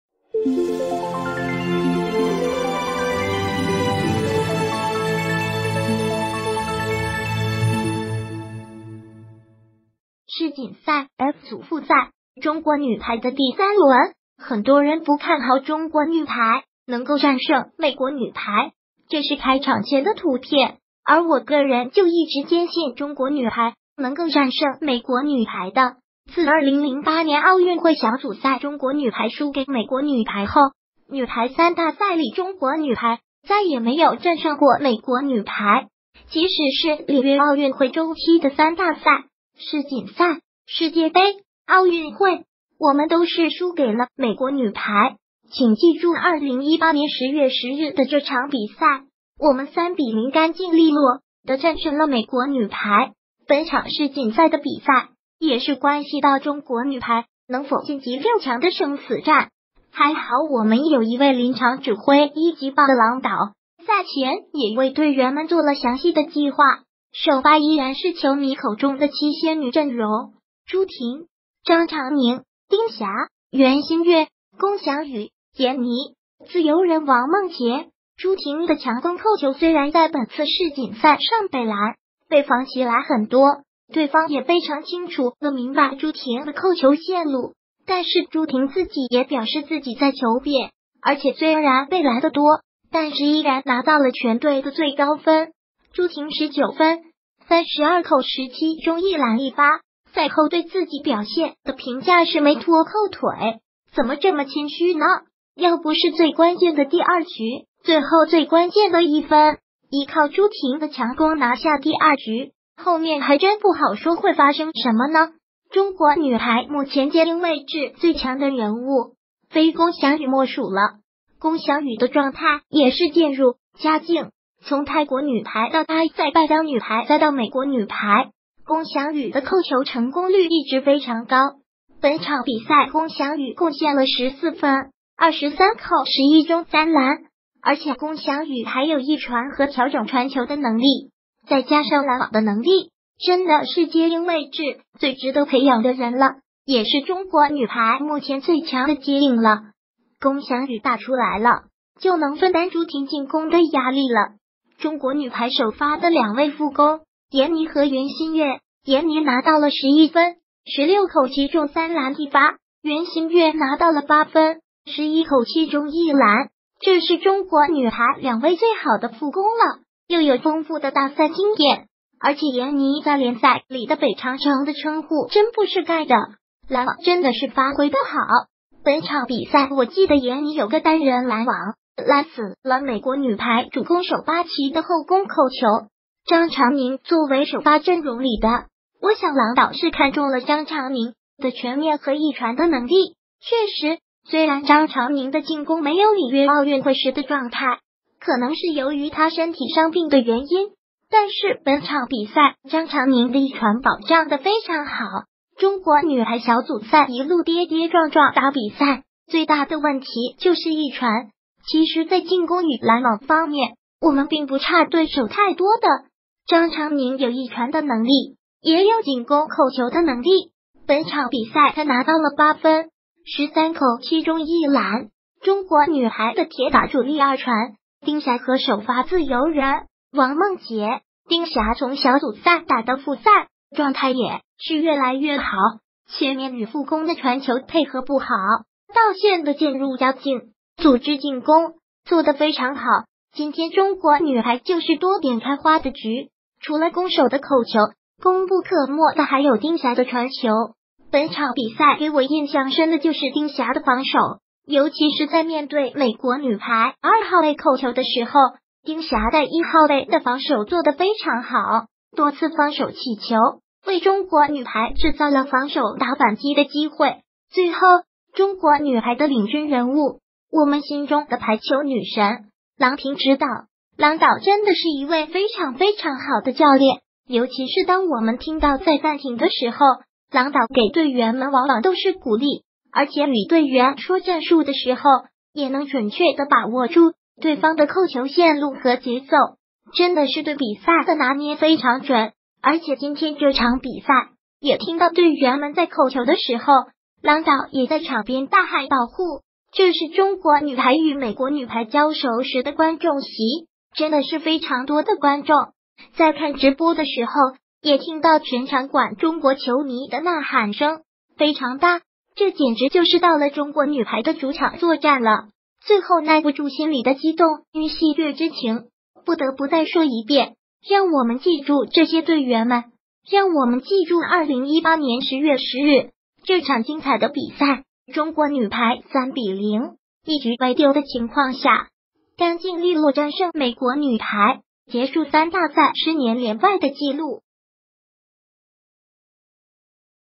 中国女排干净利落战胜美国女排，结束三大赛10年连败的纪录！
世锦赛F组复赛，中国女排的第三轮，很多人不看好中国女排能够战胜美国女排，这是开场前的图片，而我个人就一直坚信中国女排能够战胜美国女排的。自2008年奥运会小组赛中国女排输给美国女排后，女排三大赛里中国女排再也没有战胜过美国女排。即使是里约奥运会周期的三大赛，世锦赛，世界杯，奥运会，我们都是输给了美国女排。请记住2018年10月10日的这场比赛，我们3比0干净利落的战胜了美国女排。    本场世锦赛的比赛，也是关系到中国女排能否晋级6强的生死战。还好我们有一位临场指挥一级棒的郎导，赛前也为队员们做了详细的计划。首发依然是球迷口中的七仙女阵容，朱婷，张常宁，丁霞，袁心玥，龚翔宇，颜妮，自由人王梦洁。    朱婷的强攻扣球虽然在本次世锦赛上被拦，被防起来很多，对方也非常清楚的明白朱婷的扣球线路，但是朱婷自己也表示自己在求变，而且虽然被拦得多，但是依然拿到了全队的最高分，朱婷19分，32扣17中1拦1发。赛后对自己表现的评价是没拖后腿，怎么这么谦虚呢，要不是最关键的第二局最后最关键的一分，依靠朱婷的强攻拿下第二局，后面还真不好说会发生什么呢？    中国女排目前接应位置最强的人物，非龚翔宇莫属